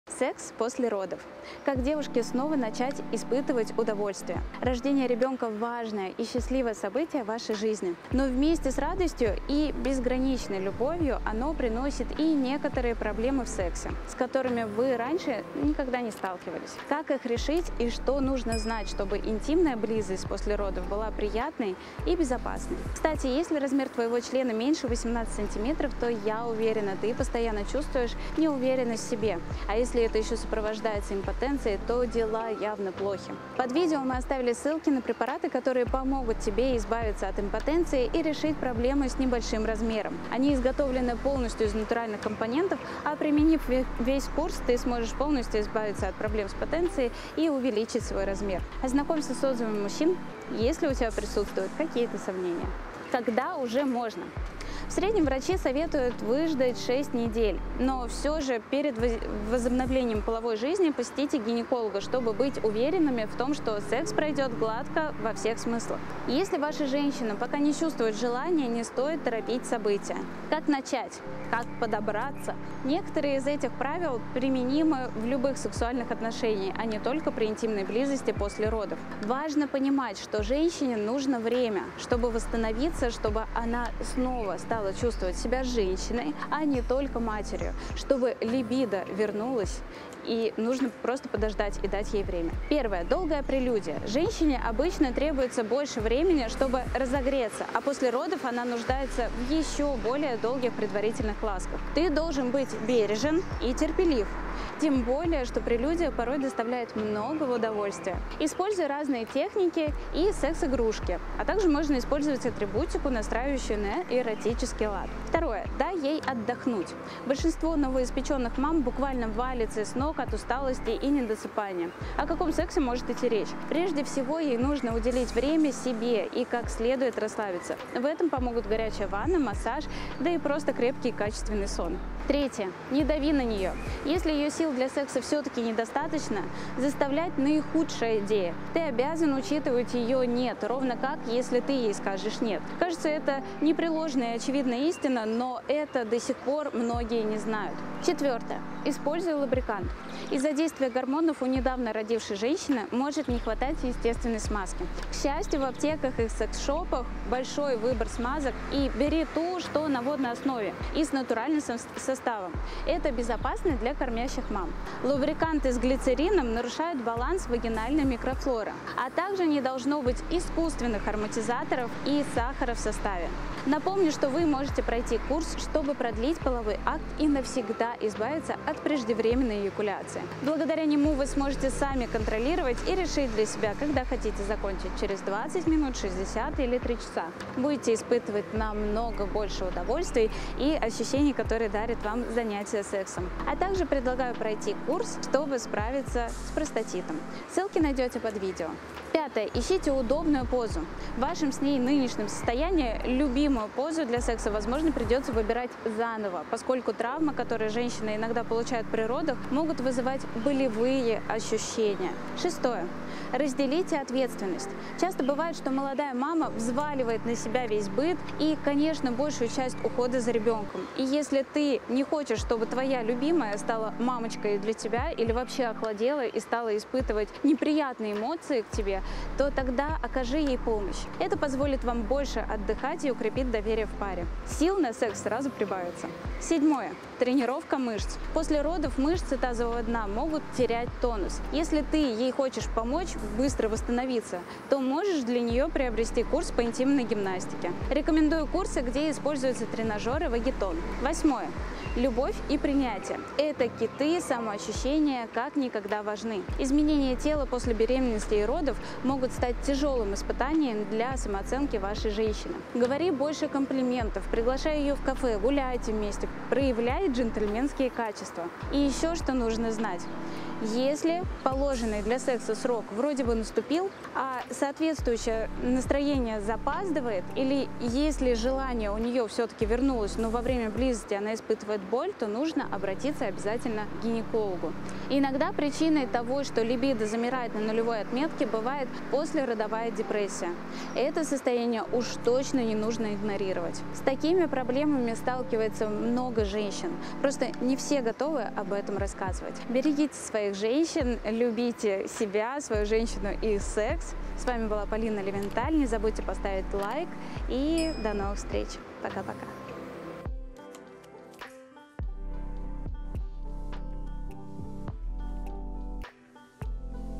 ПОЛЕЗНО ЛИ ЗАНИМАТЬСЯ СЕКСОМ ПОСЛЕ БЕРЕМЕННОСТИ?
ПОЛЕЗНО ЛИ ЗАНИМАТЬСЯ СЕКСОМ ПОСЛЕ БЕРЕМЕННОСТИ? Готов ли организм быстро приспособиться к сексу после беременности? Мы разобрали для вас достаточно важный вопрос, который волнует многих пар, можно ли заниматься сексом после беременности и, если да, то через сколько времени. Полезно ли заниматься сексом после беременности и как это делать ПРАВИЛЬНО! Сегодня в нашем видеоролике: "ПОЛЕЗНО ЛИ ЗАНИМАТЬСЯ СЕКСОМ ПОСЛЕ БЕРЕМЕННОСТИ?"